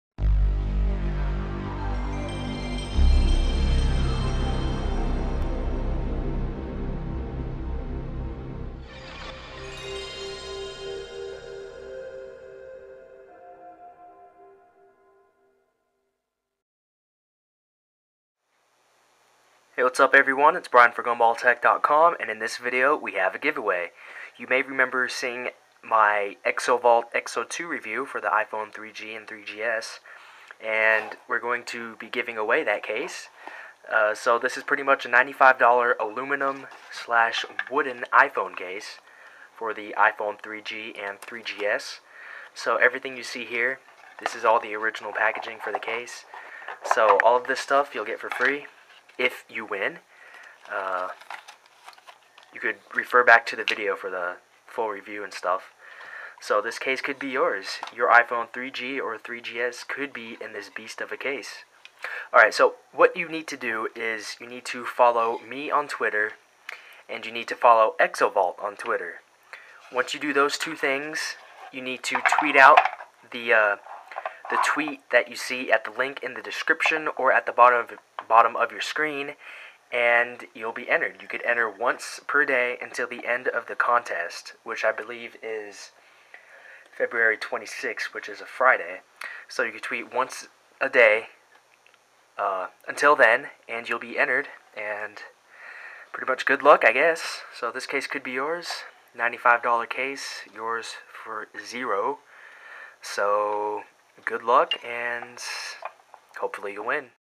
Giveaway! Exovault Aluminum iPhone 3G/3GS Case ($95 Value!)
UPDATE!
If you don't want Twitter, then you can enter by just leaving a MEANINGFUL comment for this video. Make sure it is nice and isn't spam. AND YOU MUST BE IN THE US!
UPDATE!

We're giving away the case that we reviewed (can be found It's valued at $95, so you're definitely going to have something you'll love (if you just so happen to win the contest).

There are 3 things that you need to do:
1) Follow @exovault on Twitter.
2) Follow @besweeet on Twitter.
3) Tweet the following (remember, you can do so once a day until the day the contest ends):

RT & follow @besweeet & @exovault for a chance to win a $95 exo2 iPhone 3G/3GS case! Full details

It will end on Februray 26, 2010 (Friday). You can enter once a day until that day. You must be in the US to be eligible.

Good luck!